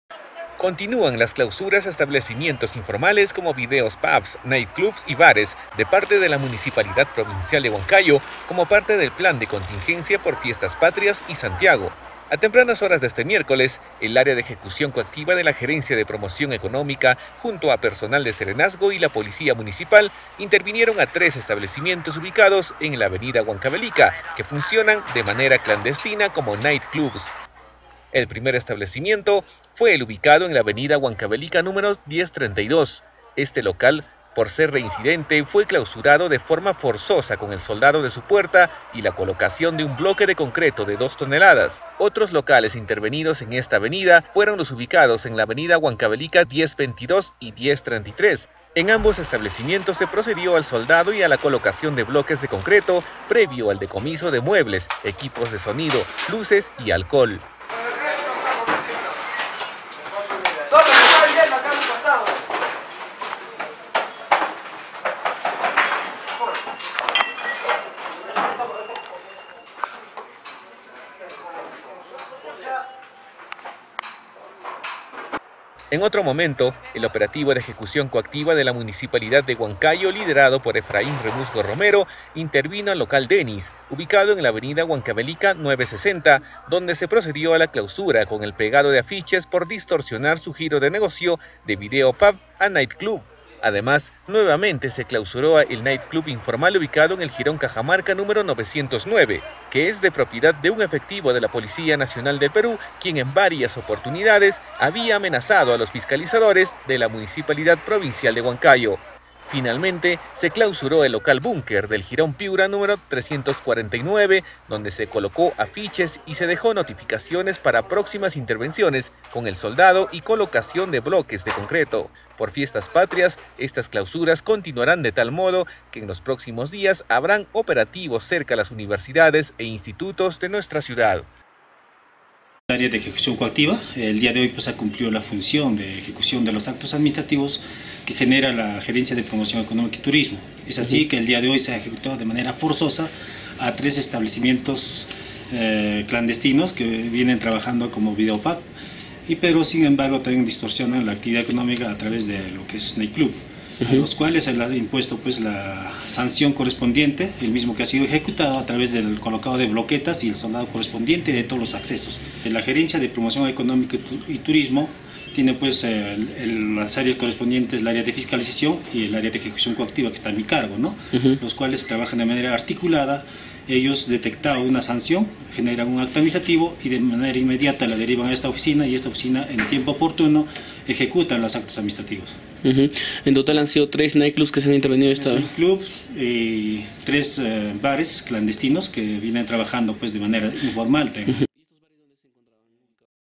SUELDAN PUERTAS A NIGTH CLUBS CADENA NOTICIAS
Tres Night Club son clausurados por funcionar de manera clandestina… a los antros reincidentes soldaron sus puertas y colocaron bloquetas de cemento.